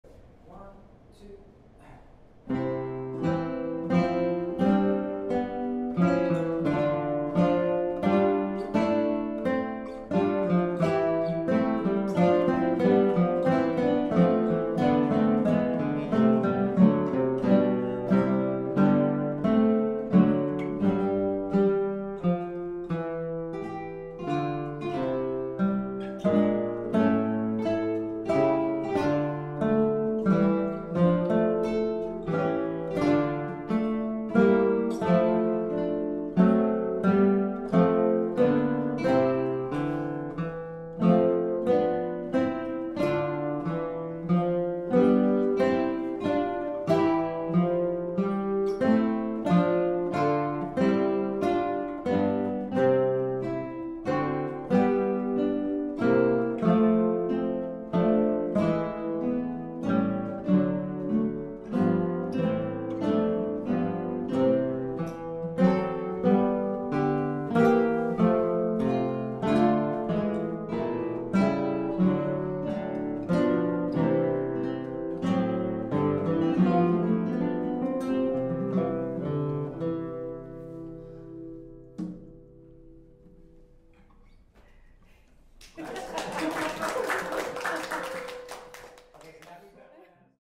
6/13 -- The Warm Up (1)
A short warm up before the show. This one is an arrangement of a famous Sor piece --do you recognize it?

From the Open Mic on June 13th, 2024